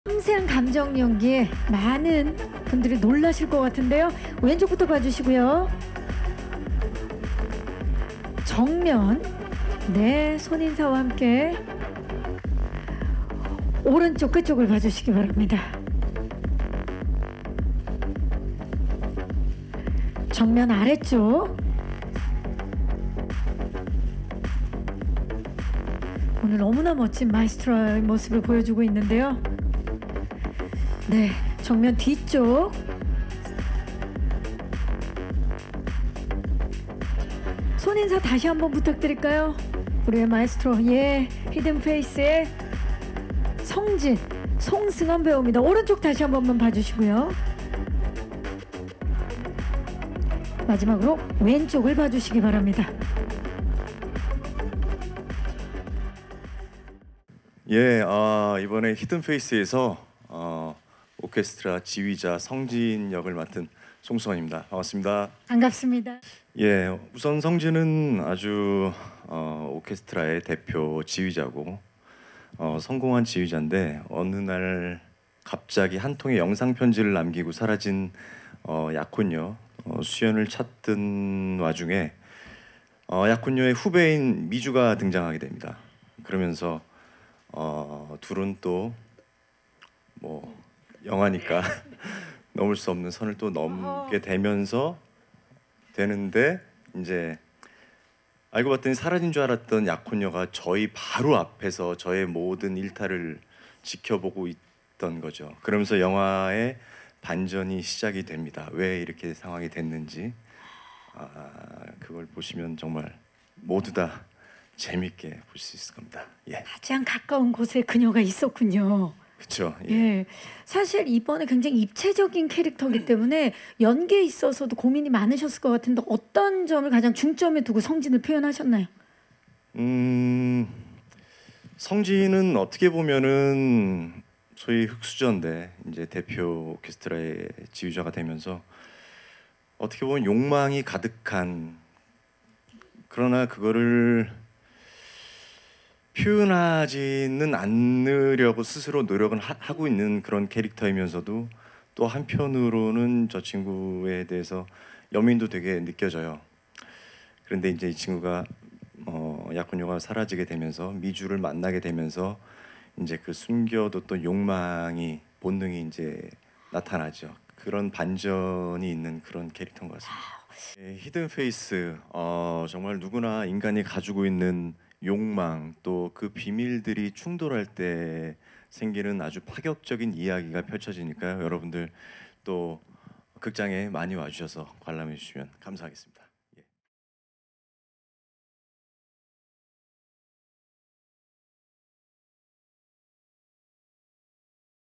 [4K] ‘히든페이스’ 송승헌, 흙수저 출신 캐릭터..숨겨진 욕망 드러내 (히든페이스 제작보고회) SongSeungheon 2024.10.22 Newsen
[뉴스엔 최재현 기자]

10월 22일 오전 서울 광진구 롯데시네마 건대입구에서 영화 히든 페이스’ 제작보고회가 열렸다.

현장에는 배우 송승헌, 조여정, 박지현, 김대우 감독이 자리해 포토타임 및 작품 관련한 토크 시간을 가졌다.

한편, 영화 '히든페이스'(감독 김대우)는 실종된 약혼녀 ‘수연’의 행방을 쫓던 ‘성진’ 앞에 ‘수연’의 후배 ‘미주’가 나타나고, 사라진 줄 알았던 ‘수연’이 그들과 가장 가까운 비밀의 공간에 갇힌 채 벗겨진 민낯을 목격하며 벌어지는 색(色)다른 밀실 스릴러 영화로 오는 11월 20일 개봉한다.

·'뉴스엔' 채널 구독하세요!